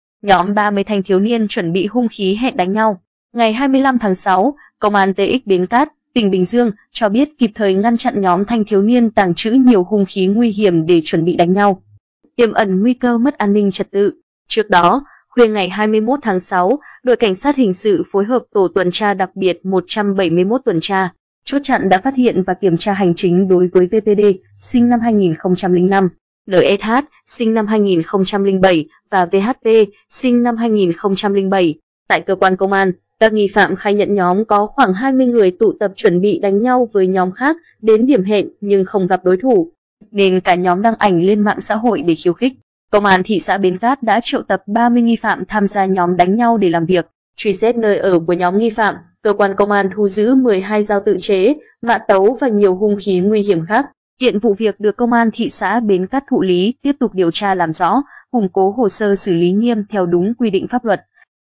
Nhóm 30 thanh thiếu niên chuẩn bị hung khí hẹn đánh nhau
Ngày 25/6, Công an TX Bến Cát (tỉnh Bình Dương) cho biết kịp thời ngăn chặn nhóm thanh thiếu niên tàng trữ nhiều hung khí nguy hiểm để chuẩn bị đánh nhau, tiềm ẩn nguy cơ mất ANTT.Hiện vụ việc được Công an thị xã Bến Cát thụ lý tiếp tục điều tra làm rõ, củng cố hồ sơ xử lý nghiêm theo đúng quy định pháp luật.
Trước đó, khuya 21/6, Đội cảnh sát hình sự phối hợp Tổ tuần tra đặc biệt 171 tuần tra, chốt chặn đã phát hiện và kiểm tra hành chính đối với V.P.Đ. (SN 2005), N.S.H. (SN 2007) và V.H.P. (SN 2007).
Tại cơ quan công an, các nghi phạm khai nhận nhóm có khoảng 20 người tụ tập chuẩn bị đánh nhau với nhóm khác, đến điểm hẹn nhưng không gặp đối thủ, nên cả nhóm đăng ảnh lên mạng xã hội để khiêu khích.
Công an thị xã Bến Cát đã triệu tập 30 nghi phạm tham gia nhóm đánh nhau để làm việc. Truy xét nơi ở của nhóm nghi phạm, cơ quan công an thu giữ 12 dao tự chế, mã tấu và nhiều hung khí nguy hiểm khác.
 Nhóm nghi phạm chuẩn bị hung khí để đánh nhau.